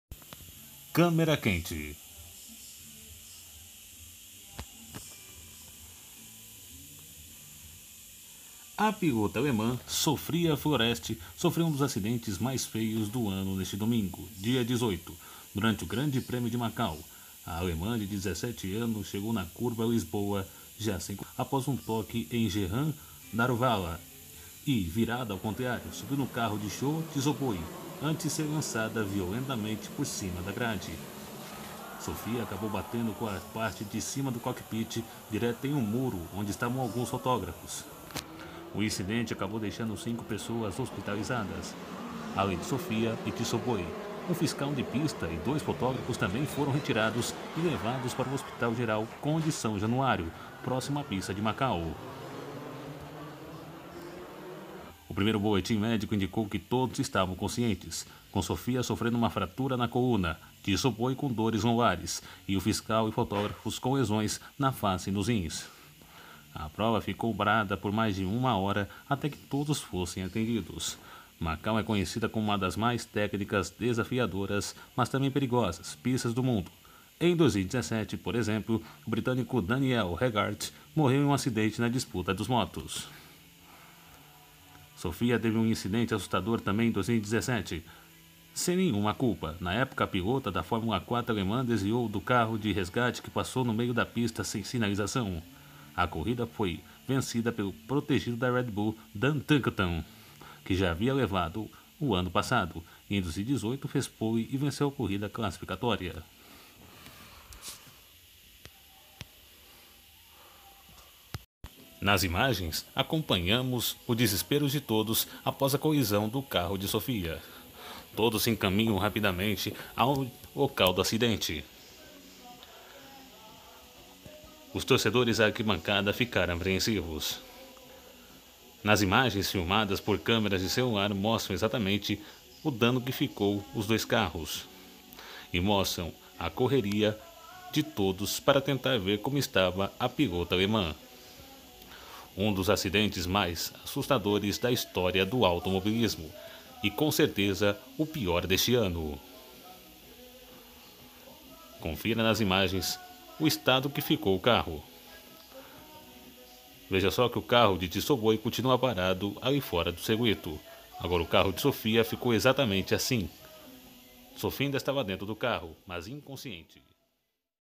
Acidente terrível da piloto Alemã Sophia Floersch
Confira o acidente detalhado da piloto Alemã Sophia Floersch, 17 anos, com a locução de Sulivan Damasceno, no Memory Tube.